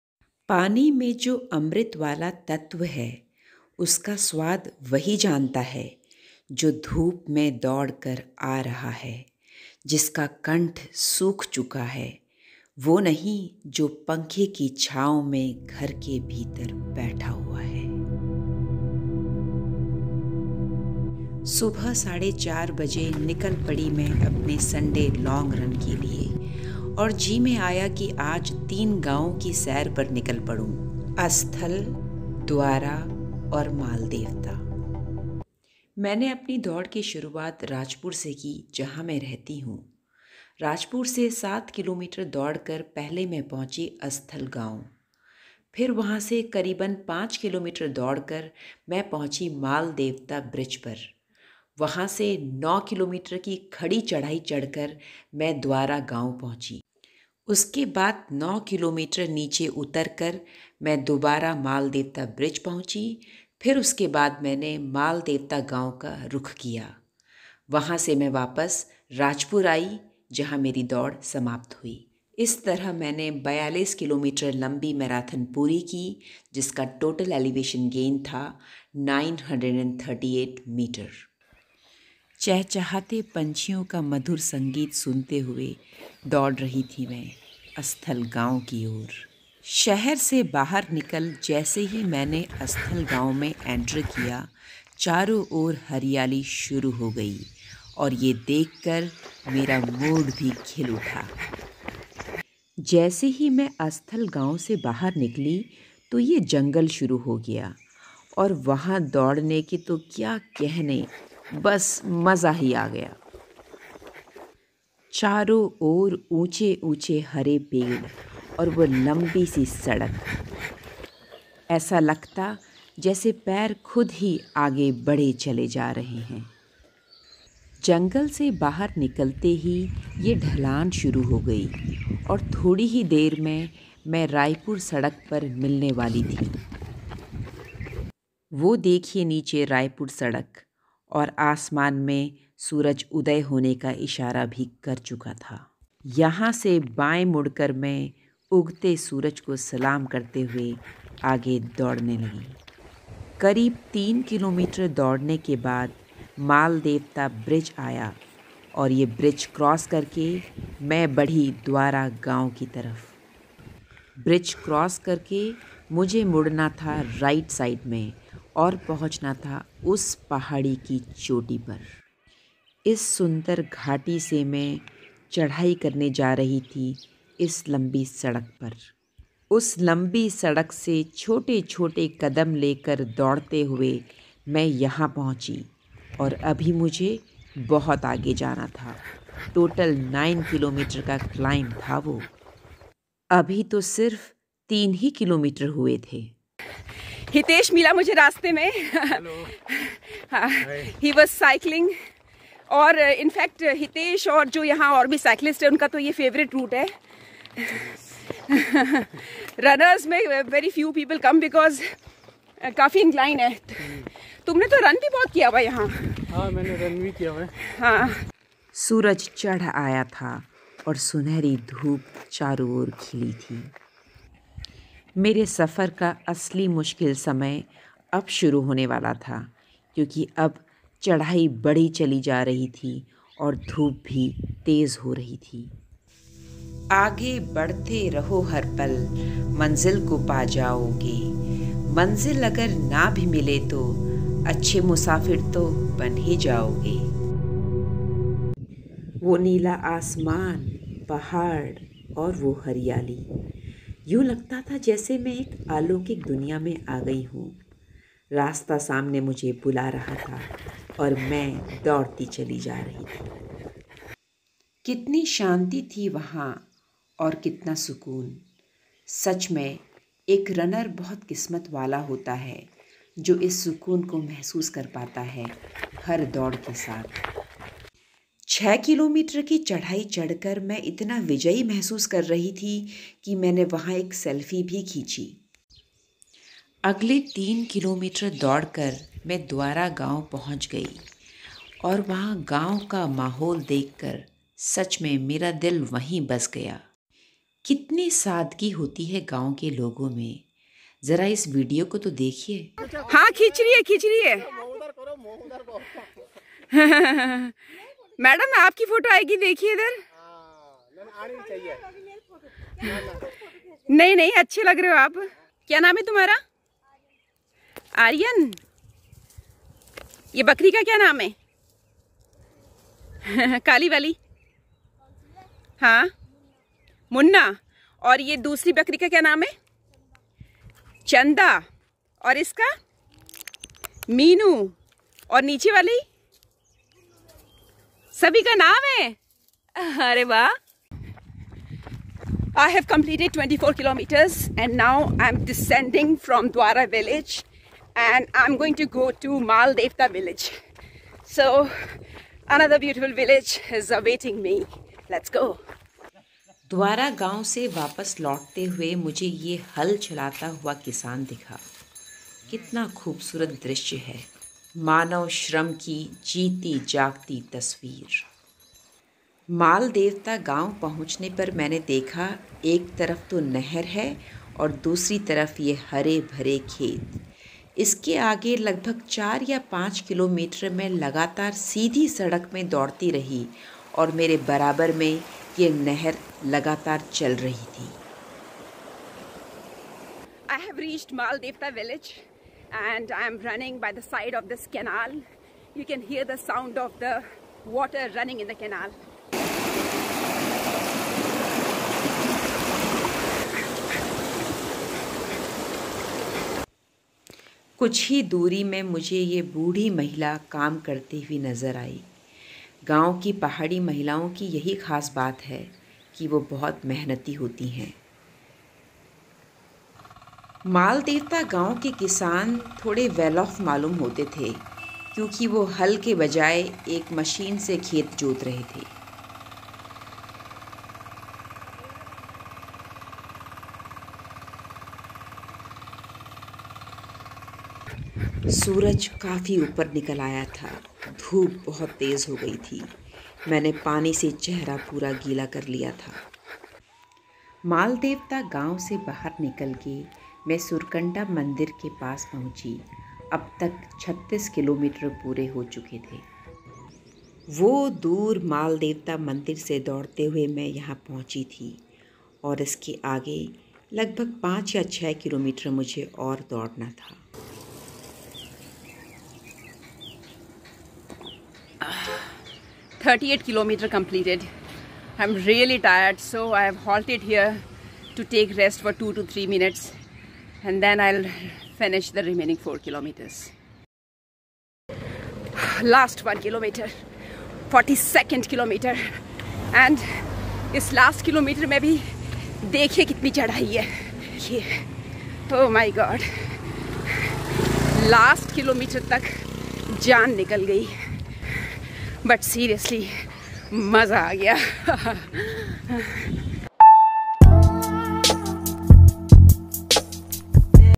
42 km दौड़ : तीन गाँवों के पार || अस्थल, द्वारा और मालदेवता
42 किलोमीटर दौड़ , 3 गांव तक - अस्थल, द्वारा और मालदेवता। खेती करते लोग, साफ़ स्वच्छ हवा और चारों ओर हरियाली। लगा जैसे 5-6 घंटे की इस दौड़ में बहुत कुछ पा लिया... जी लिया.

42 km run, up to 3 villages - Asthal, Dwara and Maldevata. Farming people, clear clean air and greenery all around. I felt like I had achieved a lot in this run of 5-6 hours….